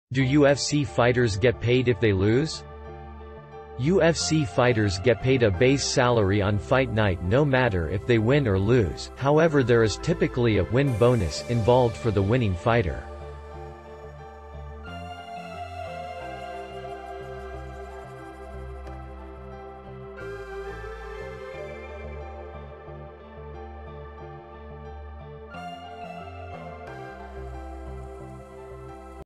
Do UFC fighters get paid if they lose?
UFC fighters get paid a base salary on fight night no matter if they win or lose, however there is typically a “win bonus” involved for the winning fighter.